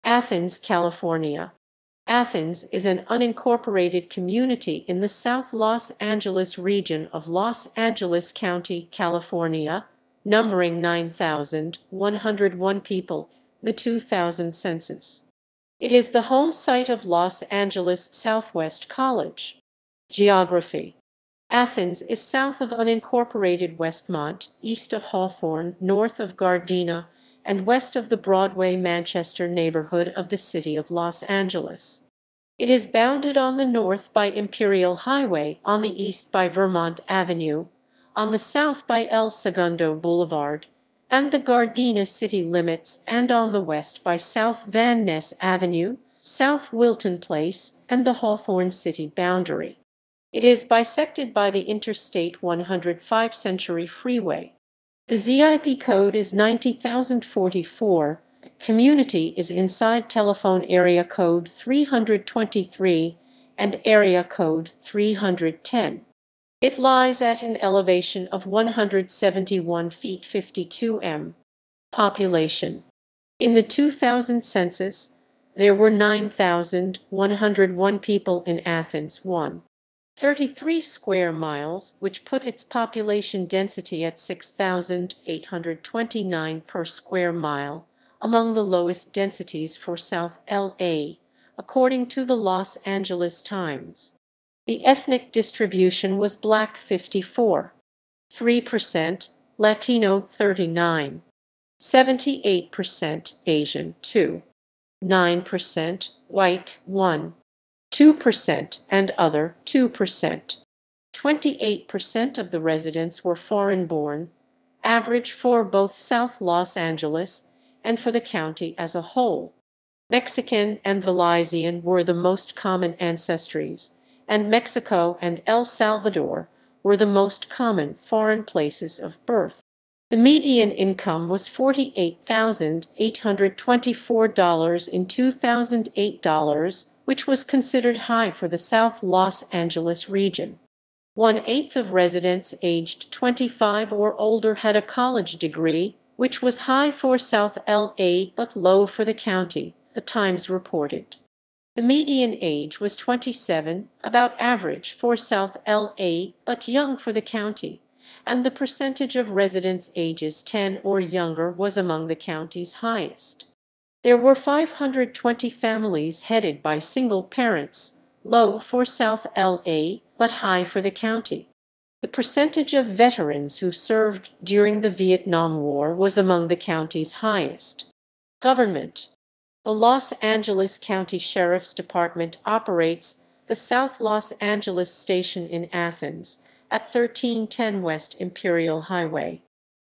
Athens, California
Athens is an unincorporated community in the South Los Angeles region of Los Angeles County, California, numbering 9,101 people in the 2000 census. It is the home site of Los Angeles Southwest College.